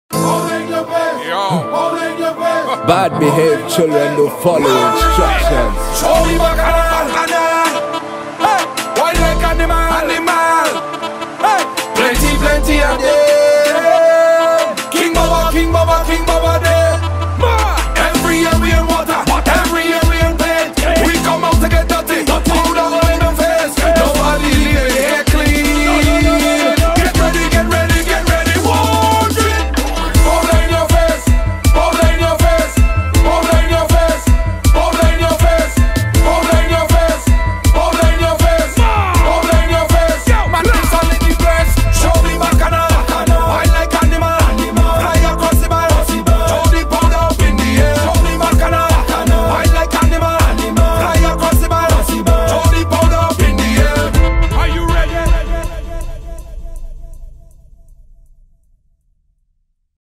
Caesars Army AM Bush Boot Camp (2024) - Barbados
Barbados GET READY to answer your Call of Duty ‼️

The BOOT CAMP is here  🇧🇧

This is the ULTIMATE test of skill, endurance and camaraderie where only the strong will survive.

Thank you very much for listening and watching please give your feedback. They will definitely inspire us to create more.